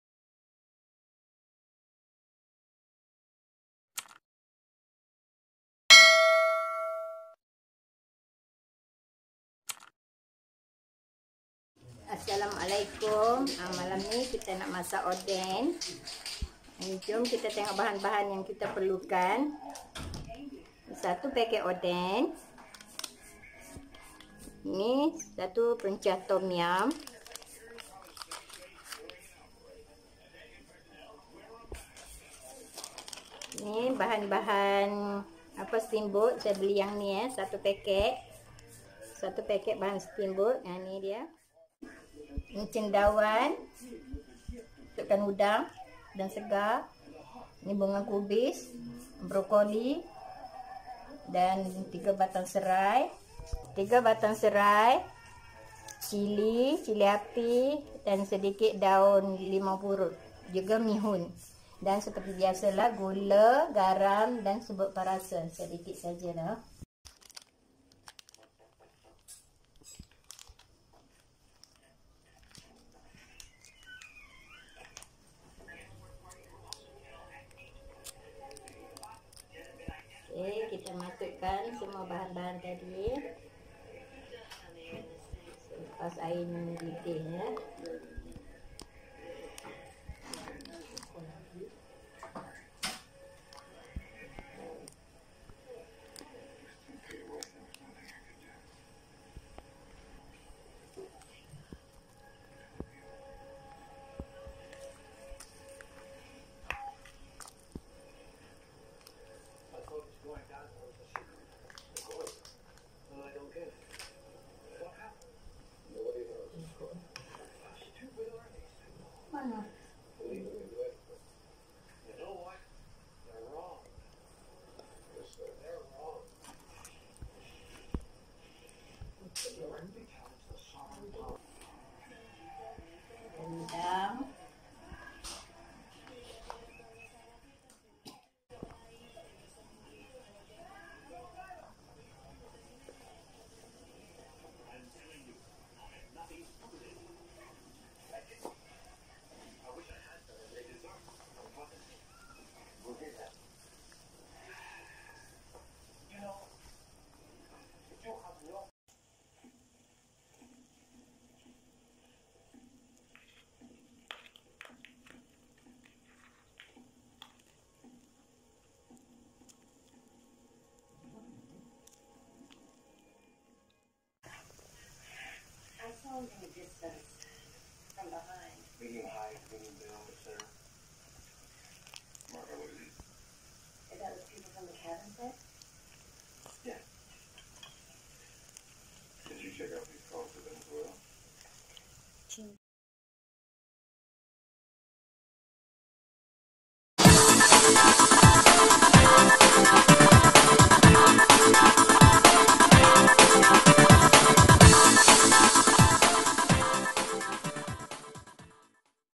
Masak Oden TomYam Cara Saya | mudah je resepi
Masak sendiri oden lebih sedap dari beli di kedai, ianya menepati citarasa sendir. Cepat dan jimat

Bahan-bahan :
Pes Oden - 1 paket
Pes tomyan - 1 packet
Steambot campur - 1 paket
Cili api - 5 biji
Cendawan - 1 paket
Brokoli - secukupnya
Bunga kubis - secukupnya
Serai - 3 batang
Daun limau purut - 5 helai
Udang - 10 ekor

Gula, garam dan ajinamoto secukup rasa